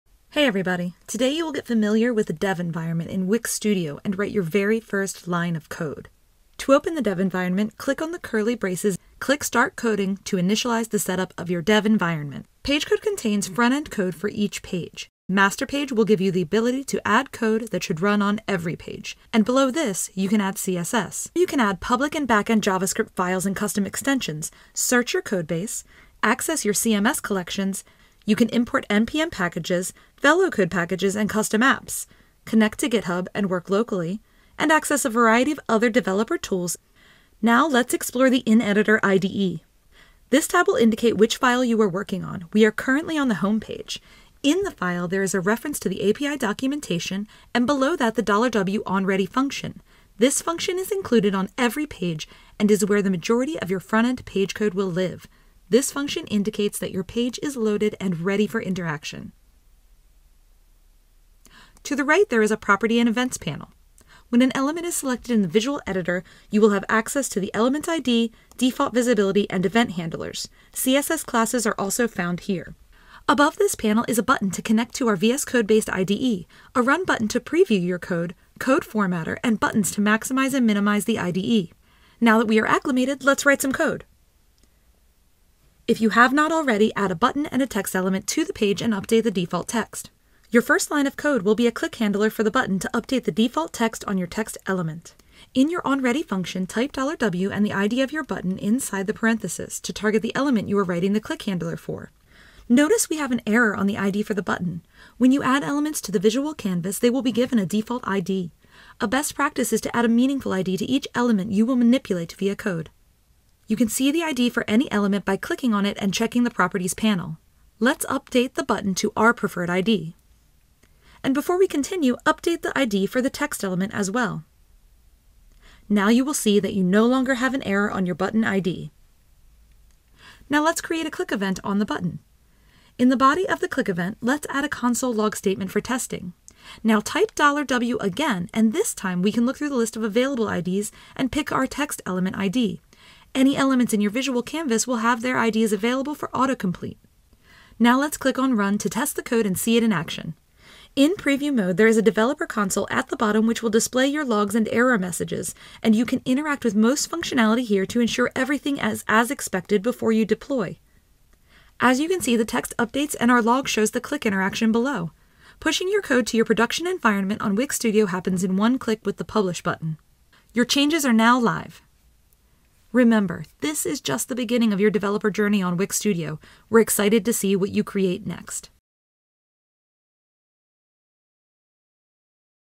How to Start Coding on Wix Studio
Get started coding on Wix Studio and write your first lines of code.

Additional Wix for Developers Resources

Description

Hello Wix Studio! In this video you will explore the developer environment on Wix Studio and get started coding by writing your first frontend event handler.

Timestamps:

00:00💡 Introduction
00:13 💼 Dev Tools Overview
00:39🛠️ Exploring the IDE
01:31 🚀 Writing a click event handler
02:30 🌐 Test and deploy your code